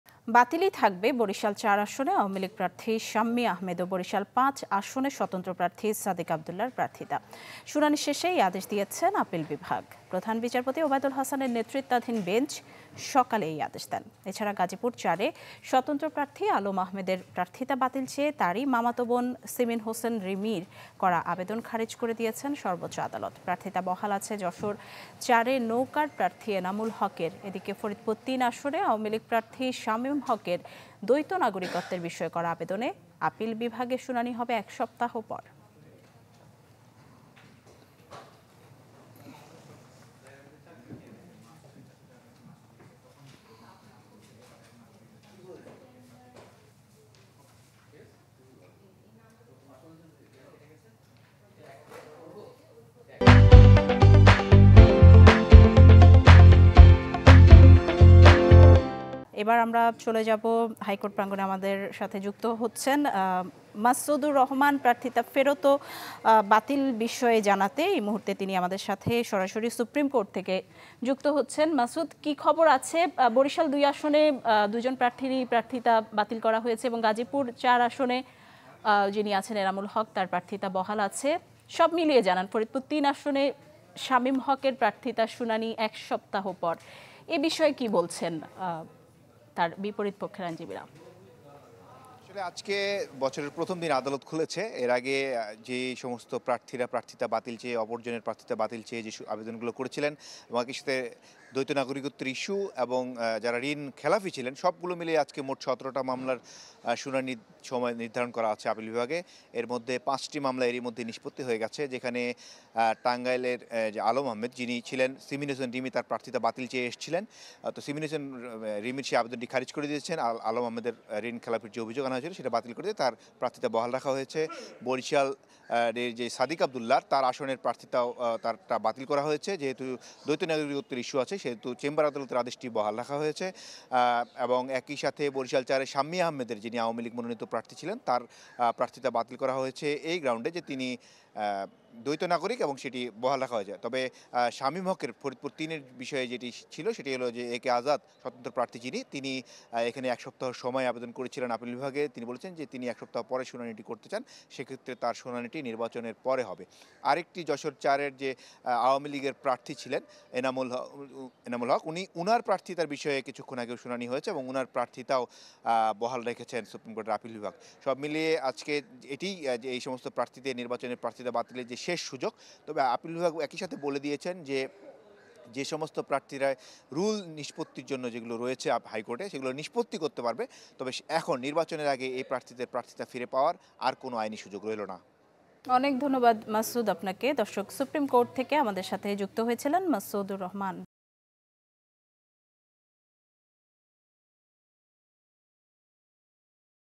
বরিশালে বাতিলই থাকবে সাদিক-শাম্মীর প্রার্থিতা: আপিল বিভাগ | Sadiq Abdullah | Shammi Ahmed | Barishal
»» Office Address:
Bangladesh.